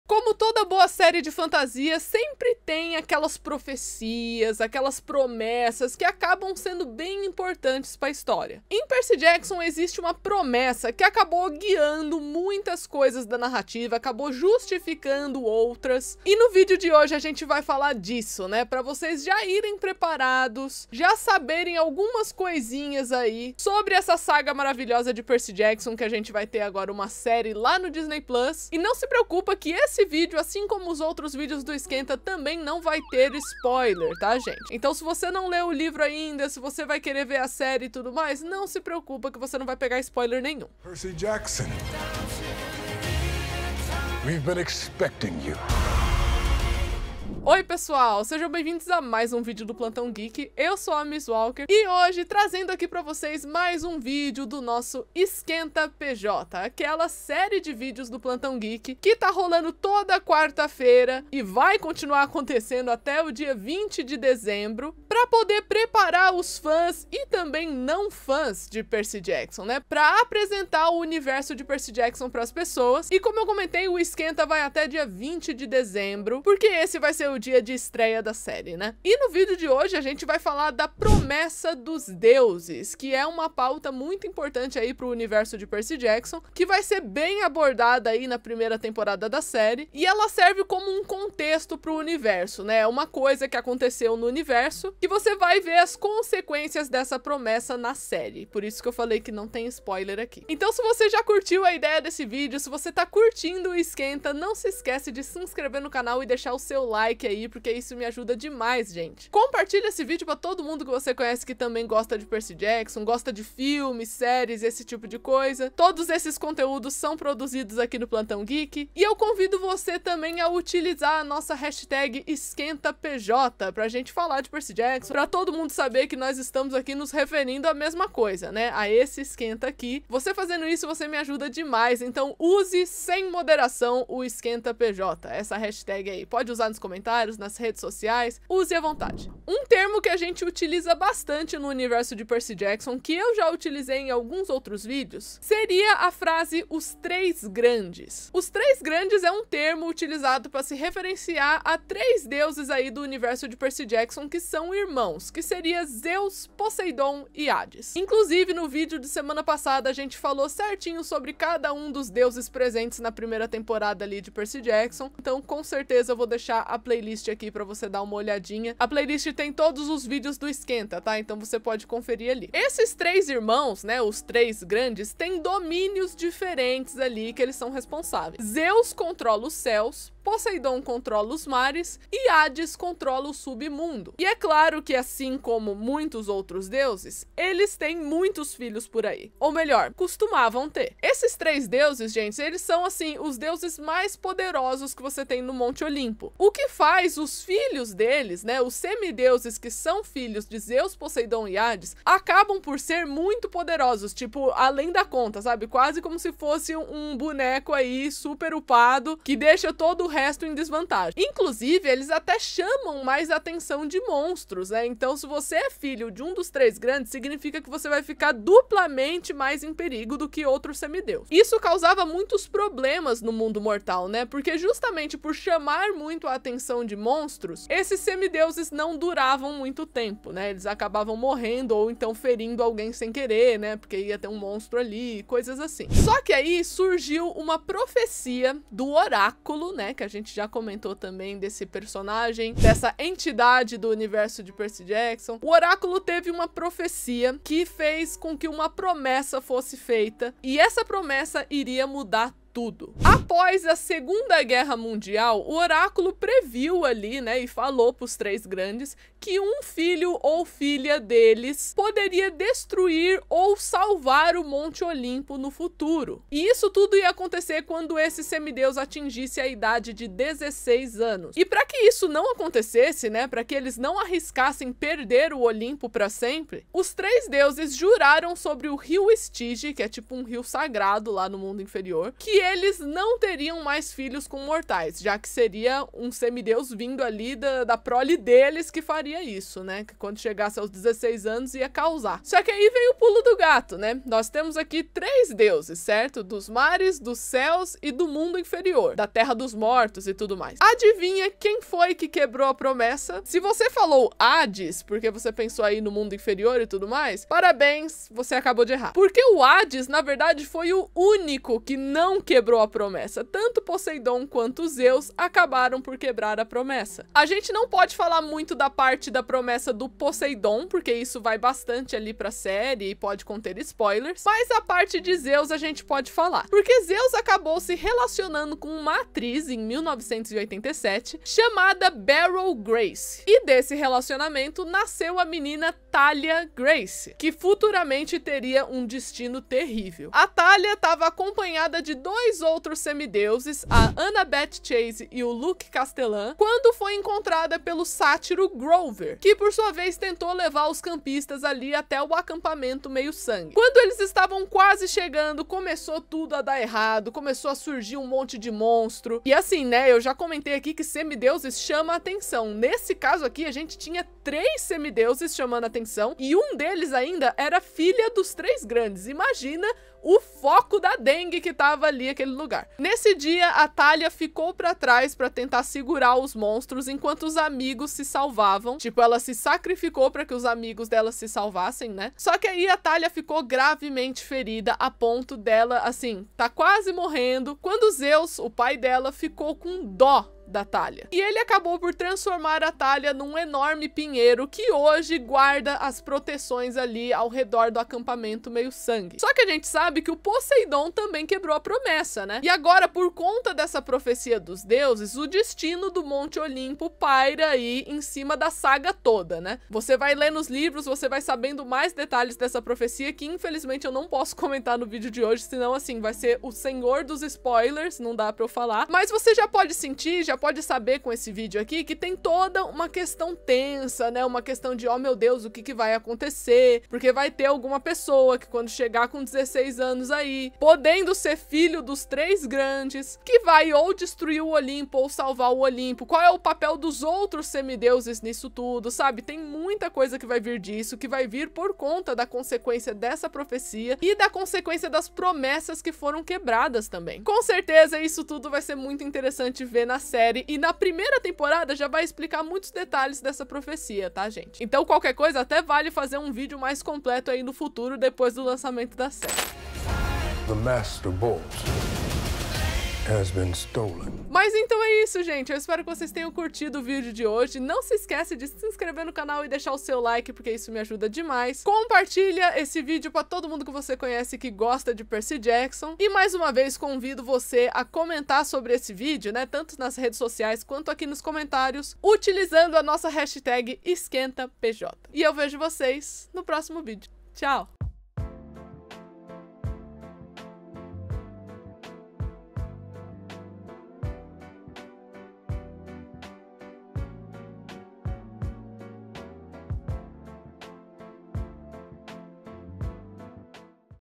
A PROMESSA DOS DEUSES 🔱 PERCY JACKSON E OS OLIMPIANOS #4
No quarto video do EsquentaPJ, o projeto que propõe apresentar o universo de PercyJackson pra quem não conhece, falei tudo que você precisa saber sobre a principal promessa de três Deuses importantes da série! Tudo sem spoiler! Bora ver?

👑 FONTES E PRODUTOS: